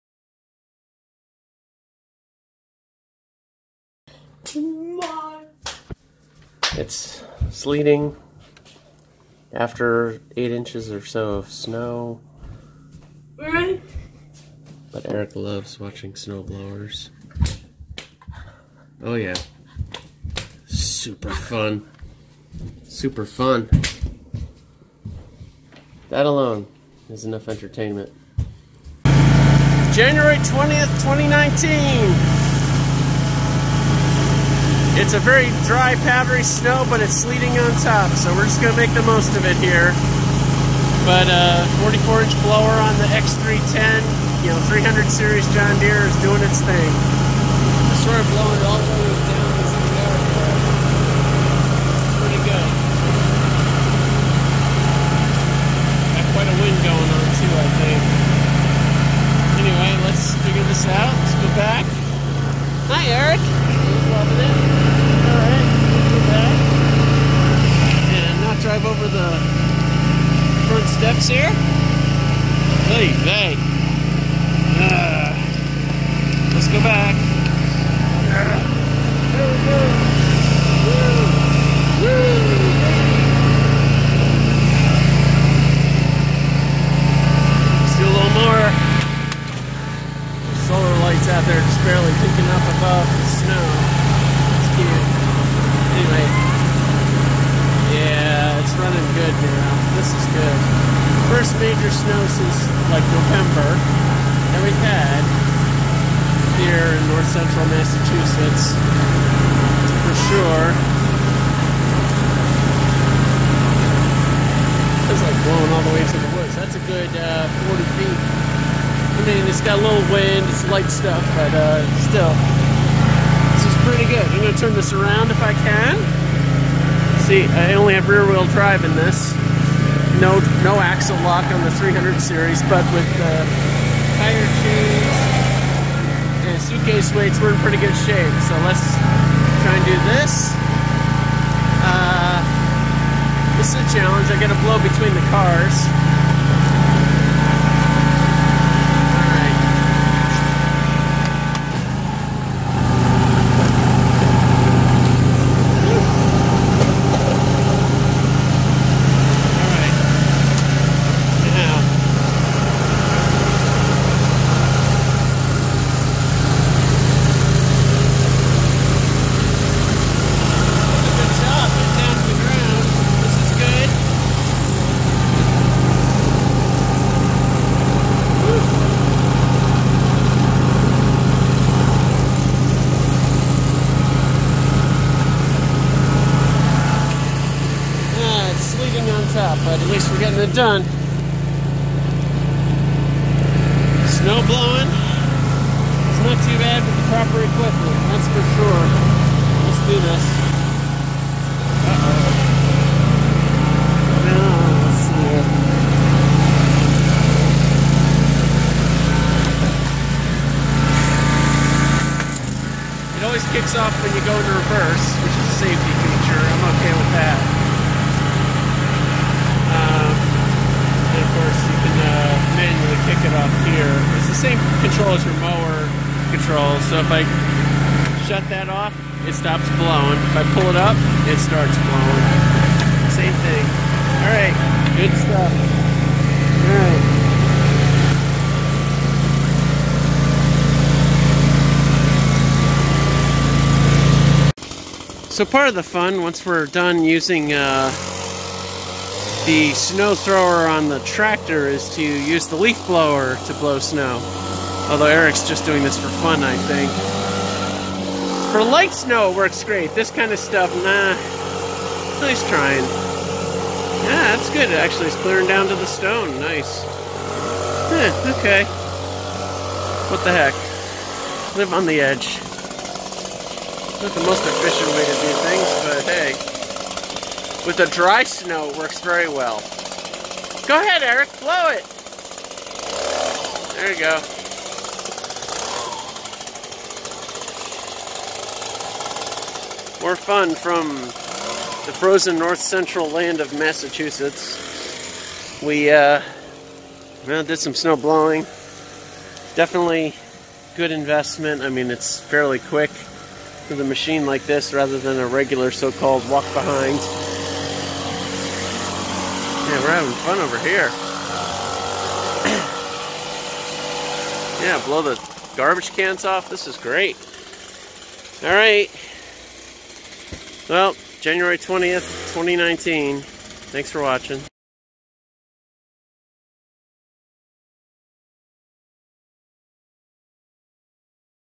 Snowblowing 20190120
Snowblowing on January 20th, 2019. We watch a neighbor do some of his driveway, then we take the John Deere x310 out with the 44 inch snowthrower. The snow was powdery, with sleet on top, so it was easy to blow.